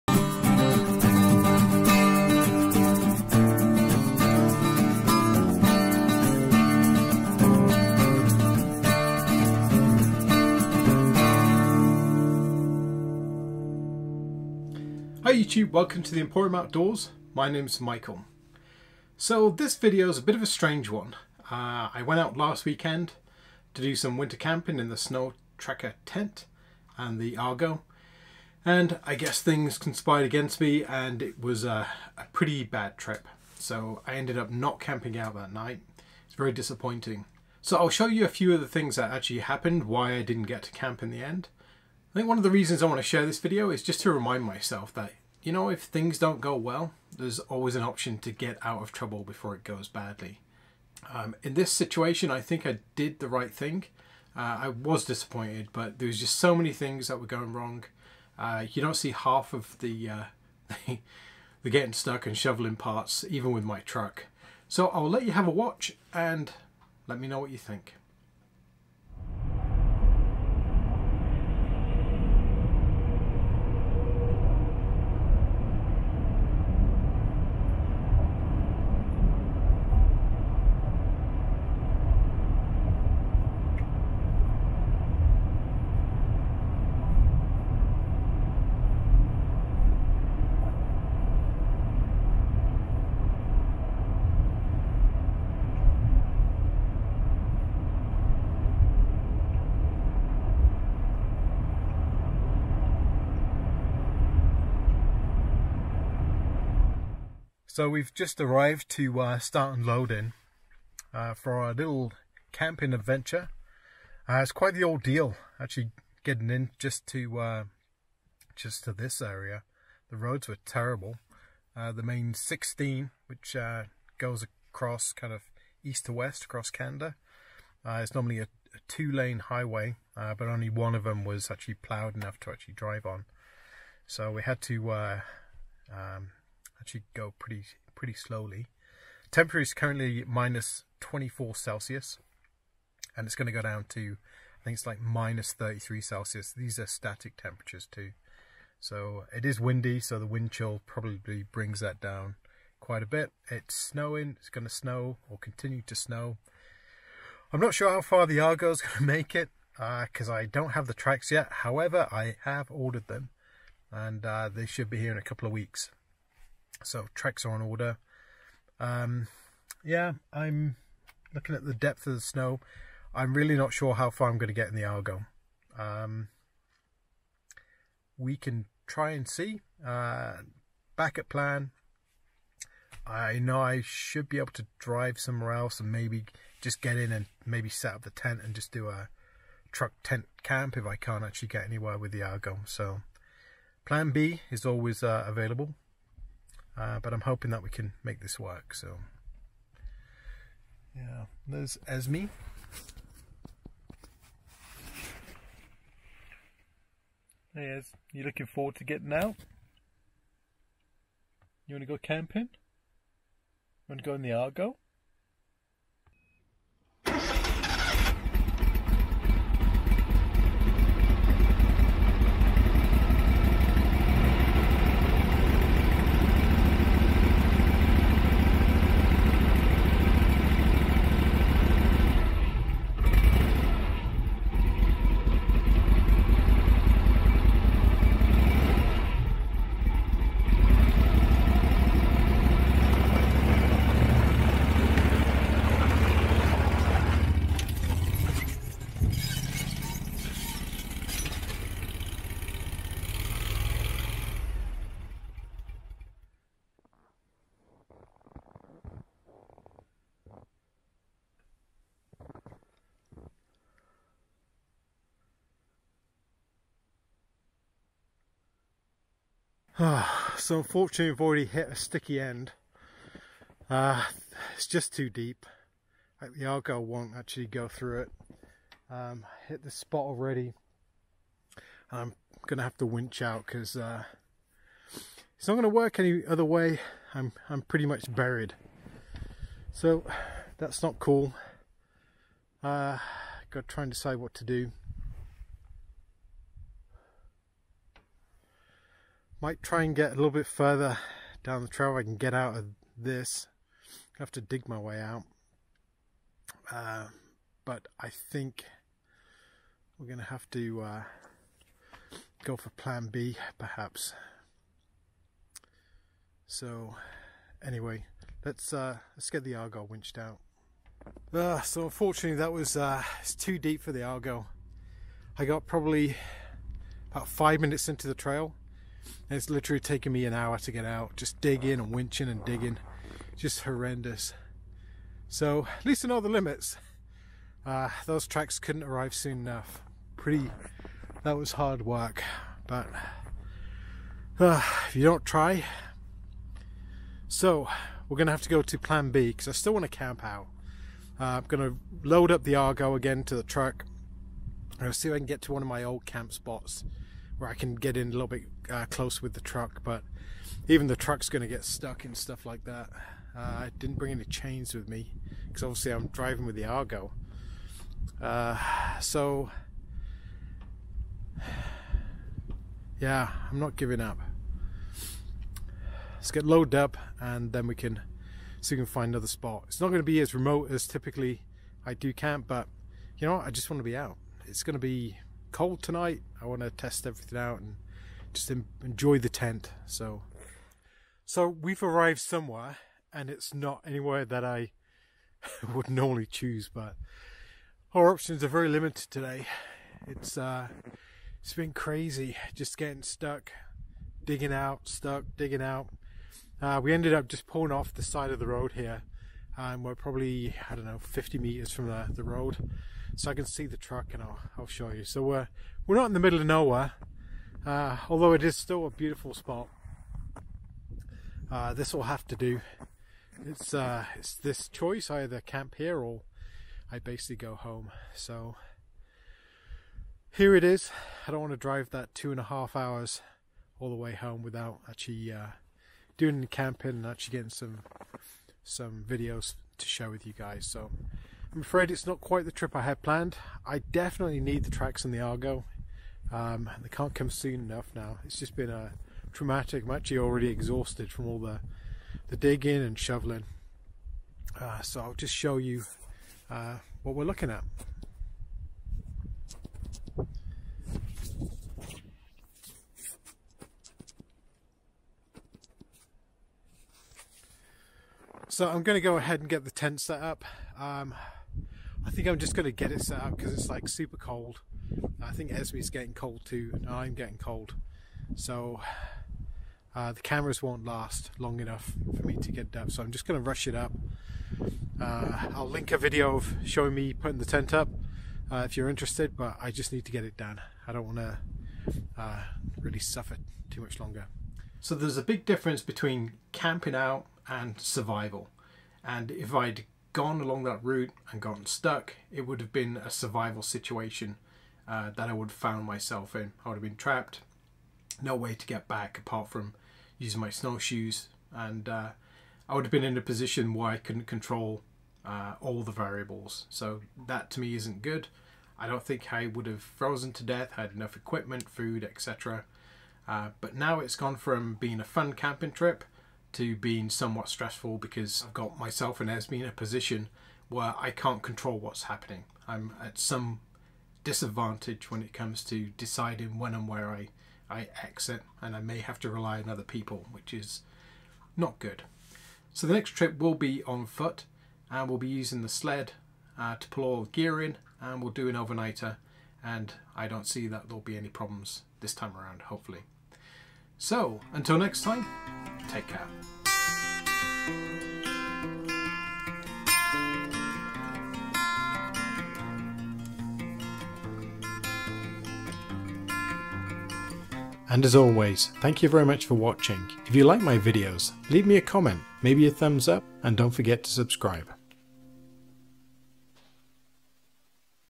Failed Winter Camping Trip - Snow Too deep for the Argo
Oh well - looking forward to the next trip!